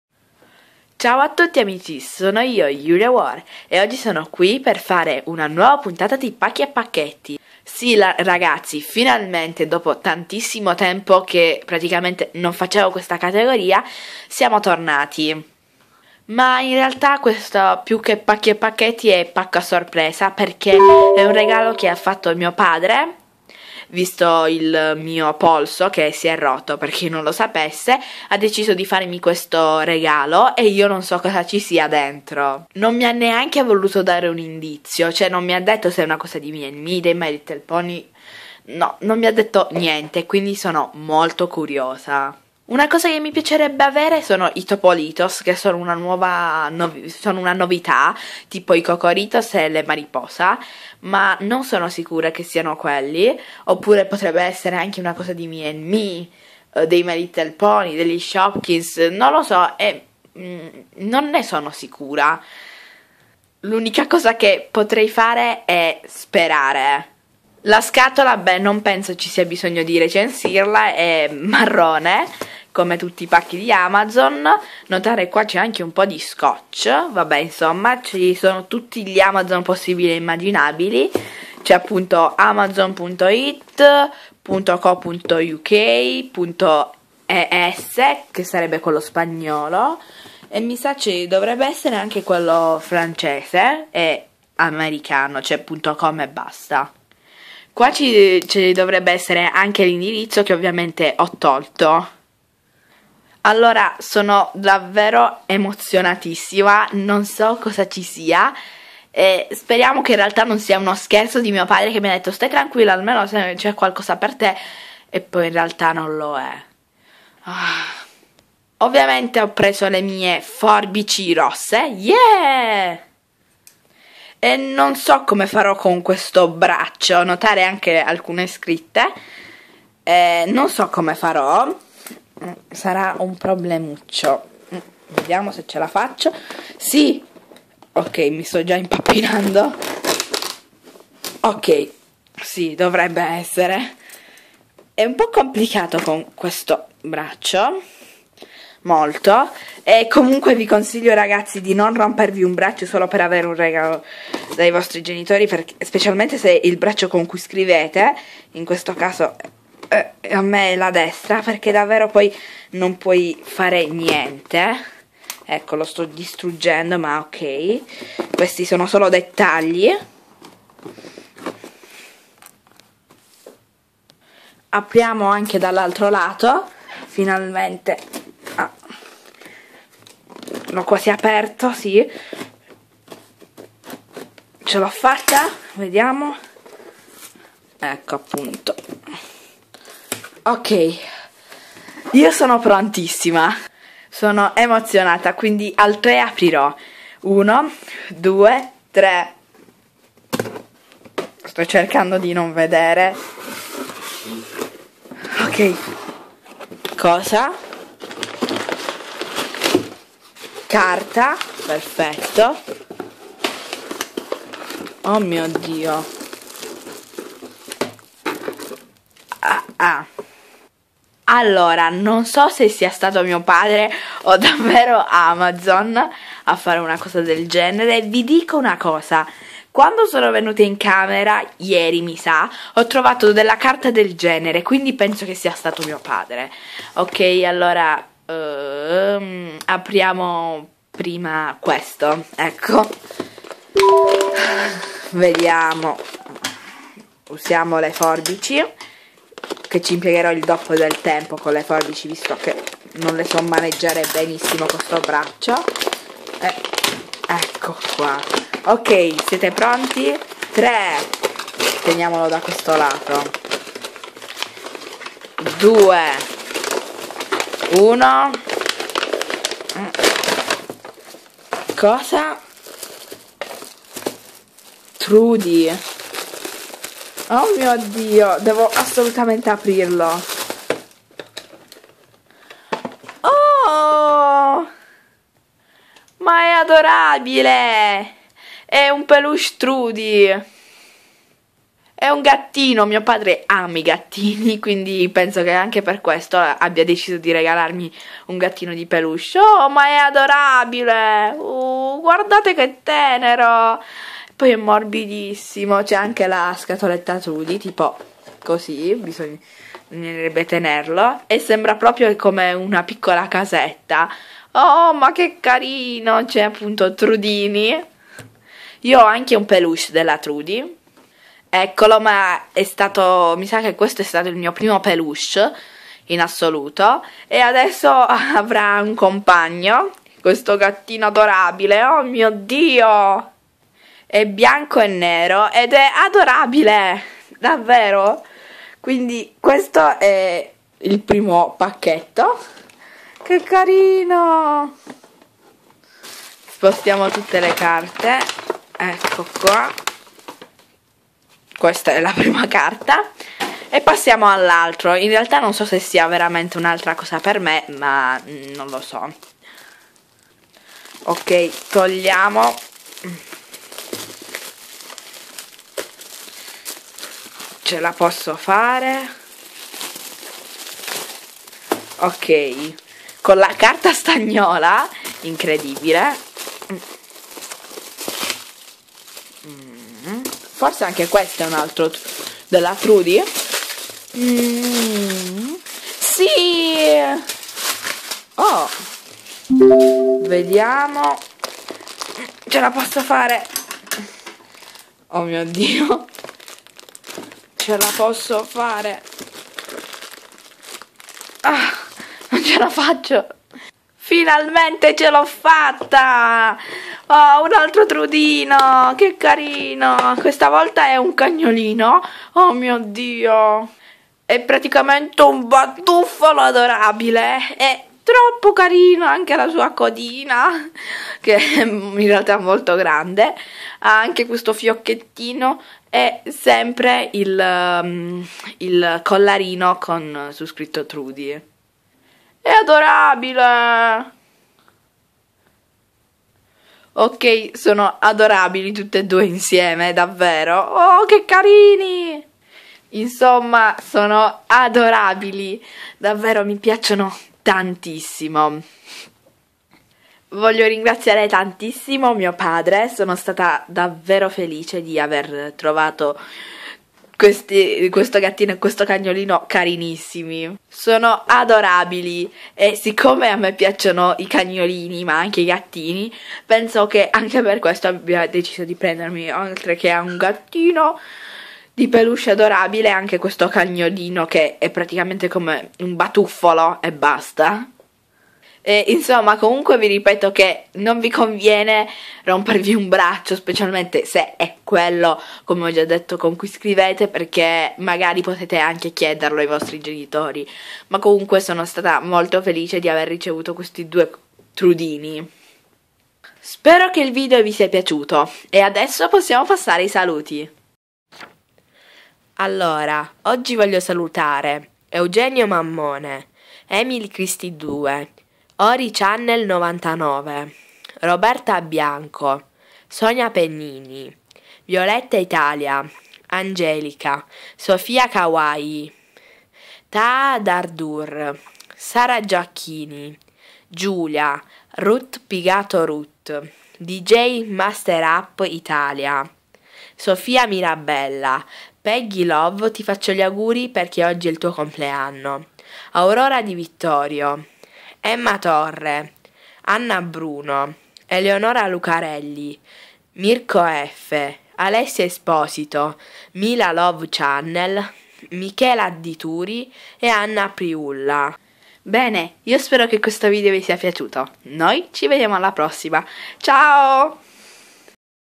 PACCO A SORPRESA
Ciao ragazzi!!! Ecco un nuovissimo video! Oggi apriremo insieme un pacco… a sorpresa! Infatti, questo è un regalo che ha deciso di farmi mio padre per tirarmi su di morale quando mi sono rotta il polso. Anche per me è una sorpresa e non vedo l’ora di aprirlo. Cosa troveremo al suo interno??? Lo scopriremo insieme. Buona visione. ❤❤❤